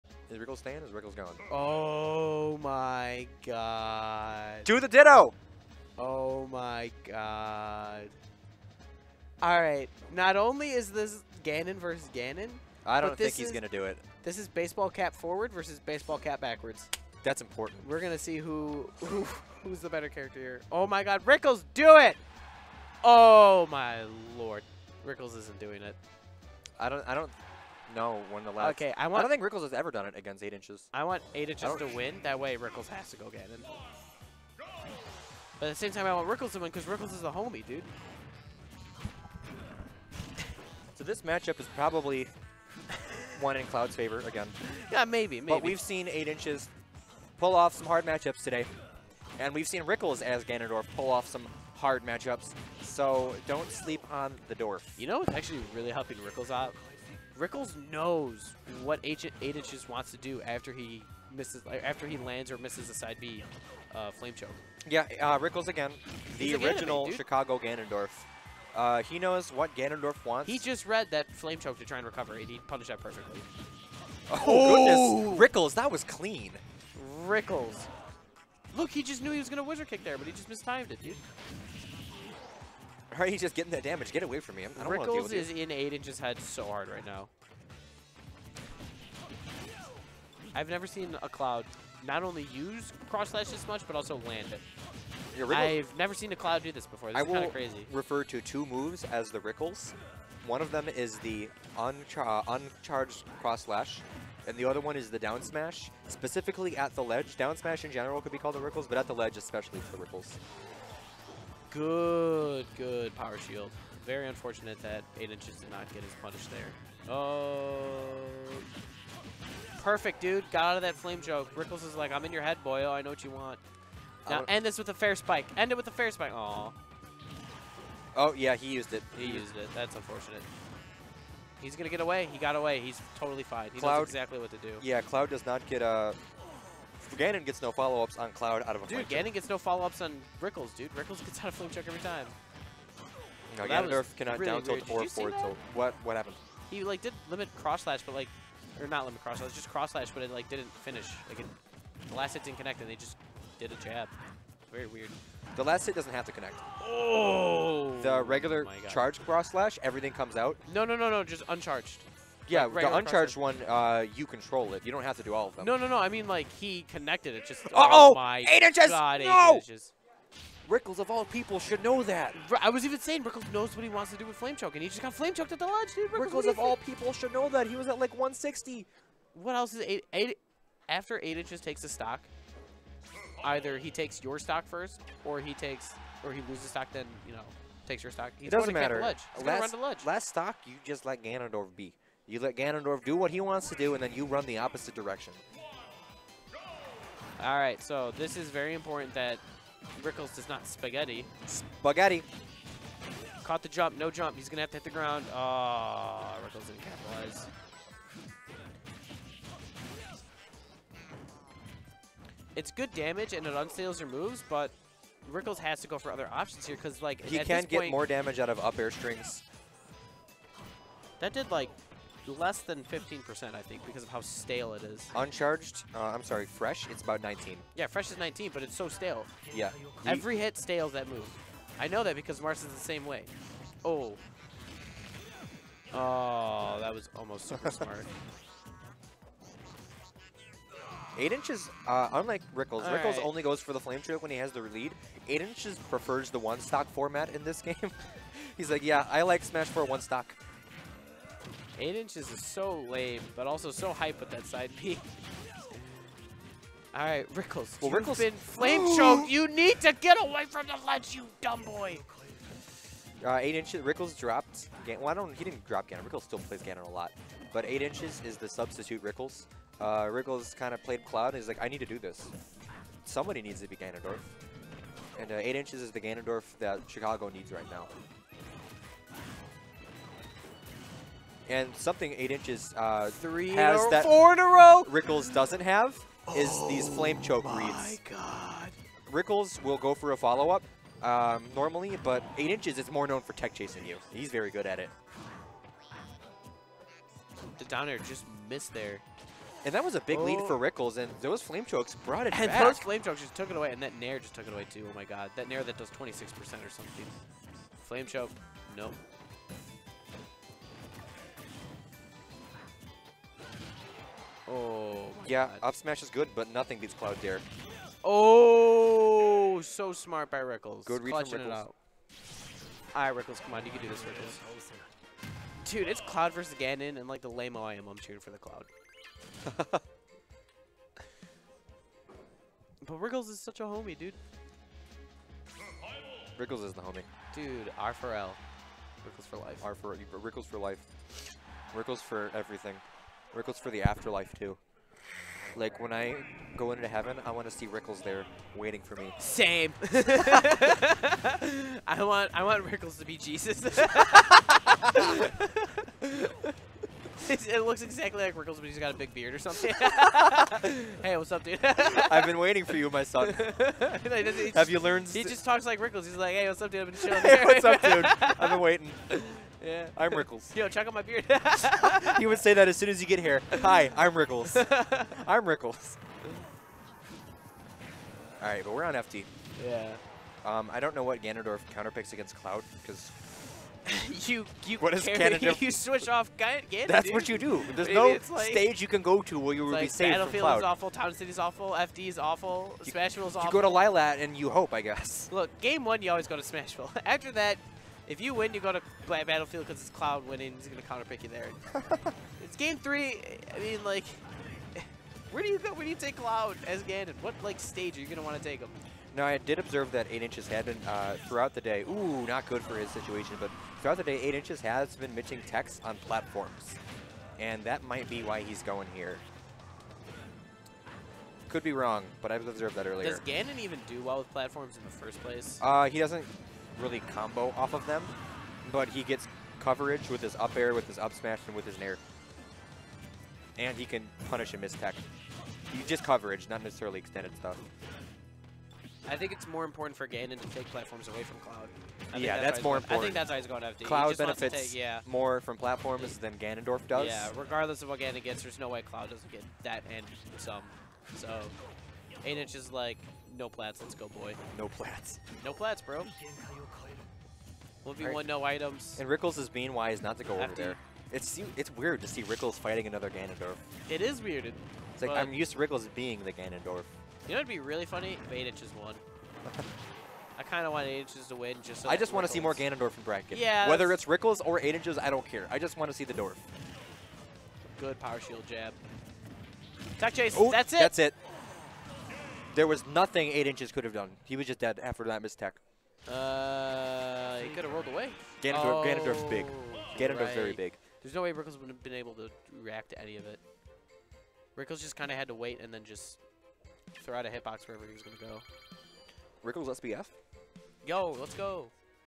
PCB #63 Losers Quarters – 8 Inches (Ganondorf) vs. E2C l Rickles (Cloud)
Poplar Creek Bowl:
2354 W Higgins Rd, Hoffman Estates, IL 60169
8/23/16